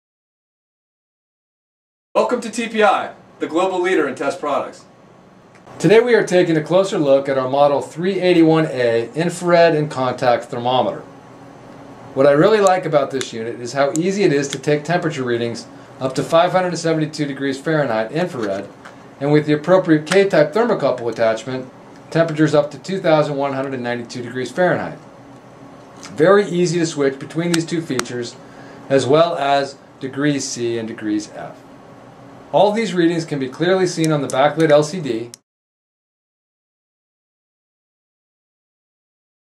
TPI 381F Infrared Thermometer: Overview
This video will serve as an overview of this infrared thermometer's main features. Additionally, it is very easy to switch between settings.